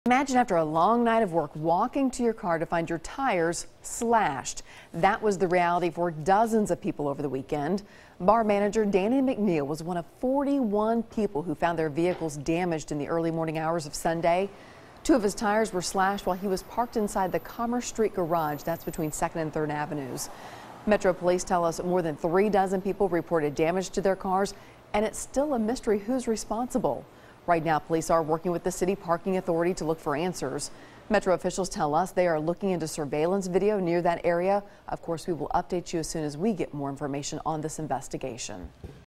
Cars Damaged In Downtown Tire Slashing
Dozens of cars were damaged in downtown Nashville when someone slashed their tires.